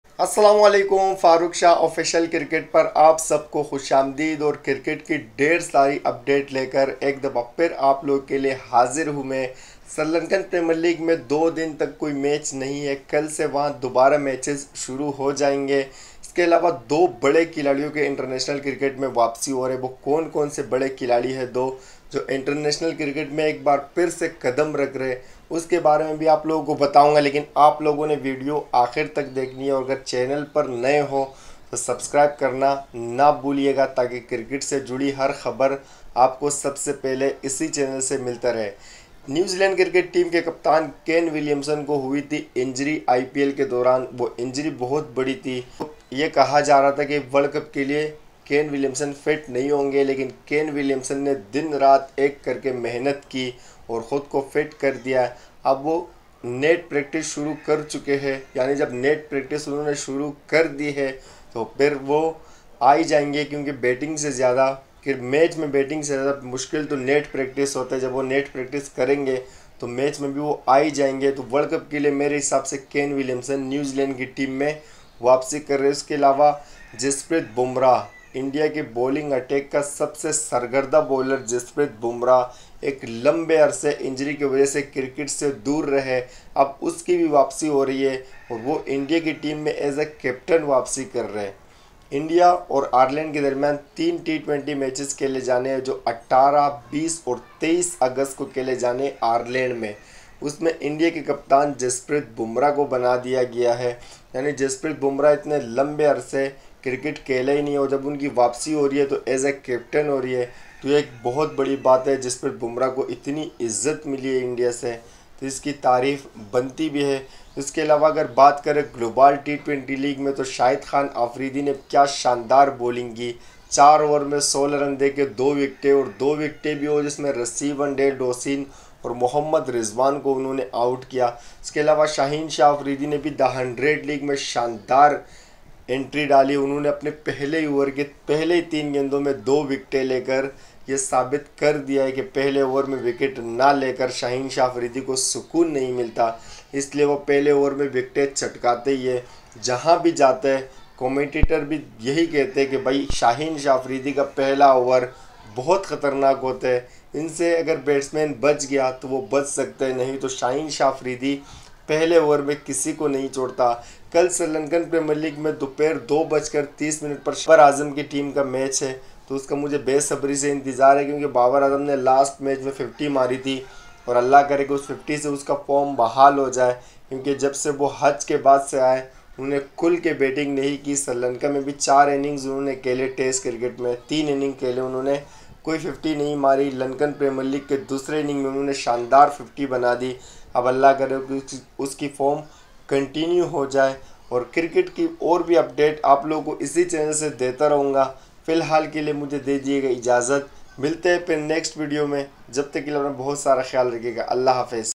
Kane Williamson and bumrah is come back in international cricket|cricket update
Jaspreet bumrah aur Kane Williamson international cricket mein wapas a chuke hain Shahid Afridi ne gt20 league mein ki shandar bowling Shaheen Afridi first over wicket#cricket